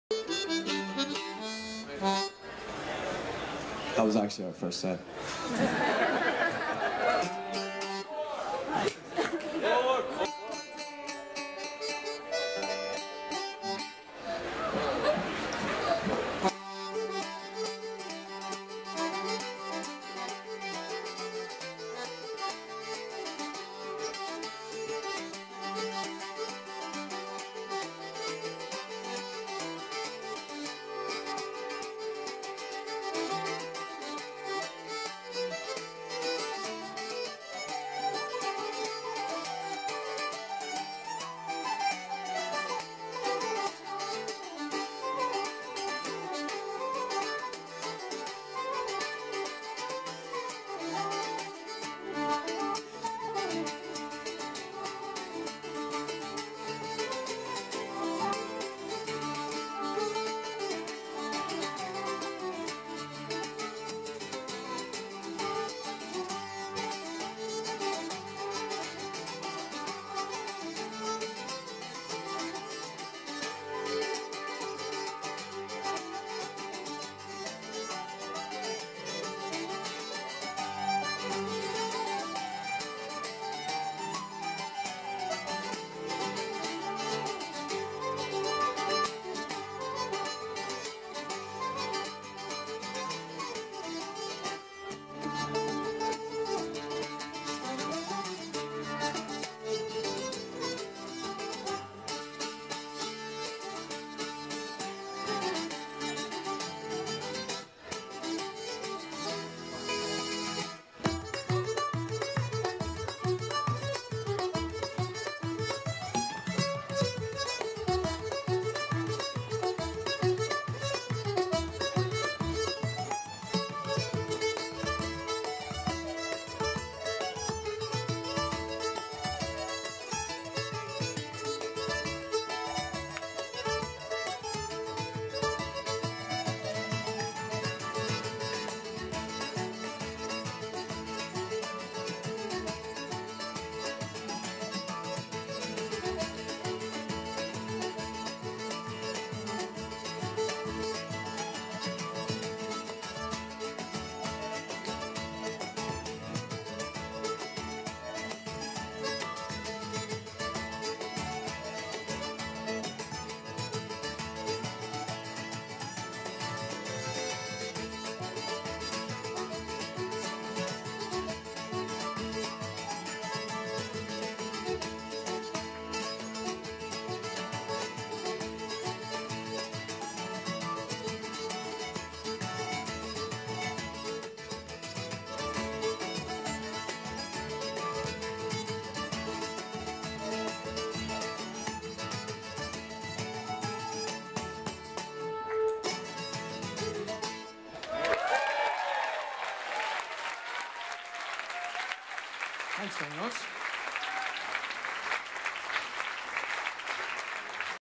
Ioscaid (Cambridge Folk Festival 2012)
Stage 2, Cambridge Folk Festival, Sunday the 29th of July 2012.

Liked these lads so much at the Festival Session on Saturday that we made of point of going to see them again on Sunday and bought their CD too.  Talented beyond their years!